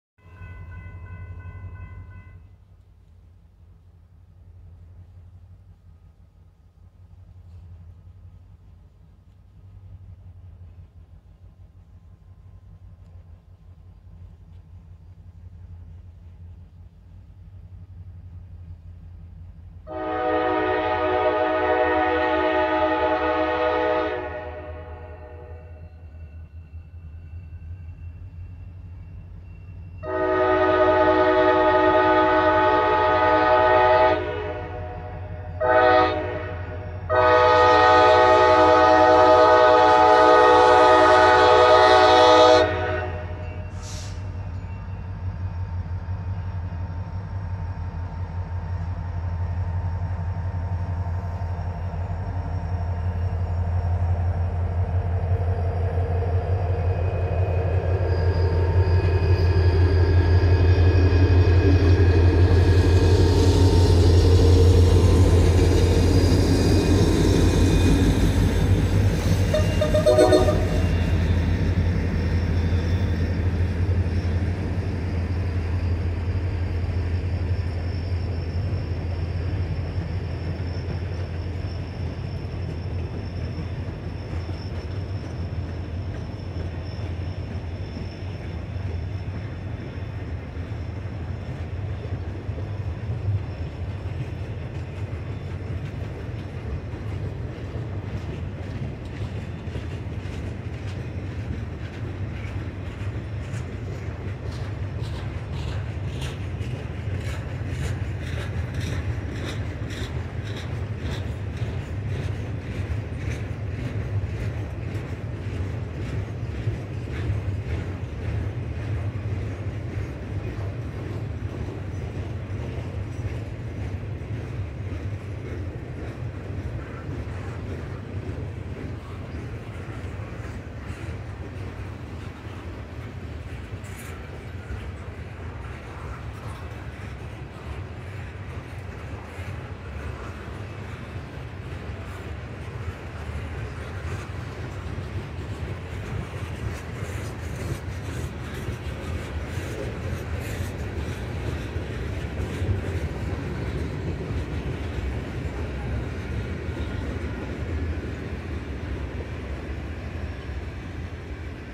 work train on the local
A785 brings a work train north to Ocala